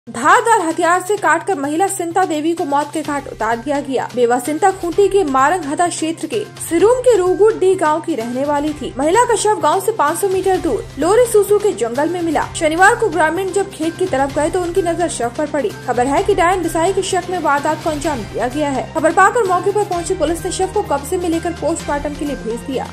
धारदार हथियार से का'ट डाला
धारदार हथियार से काटकर महिला सिनता देवी को मौत के घाट उतार दिया गया। बेवा सिनता खूंटी के मारंगहदा क्षेत्र के सिरुम के रुगुद डीह गांव की रहनेवाली थी। महिला का शव गांव से 500 मीटर दूर लोरेसुसु के जंगल में मिला। शनिवार को ग्रामीण जब खेत की तरफ गये तो उनकी नजर शव पर पड़ी। खबर है कि डायन बिसाही के शक में वारदात को अंजाम दिया गया है। खबर पाकर मौके पर पहुंची पुलिस ने शव को कब्जे में कर पोस्टमार्टम के लिए भेज दिया।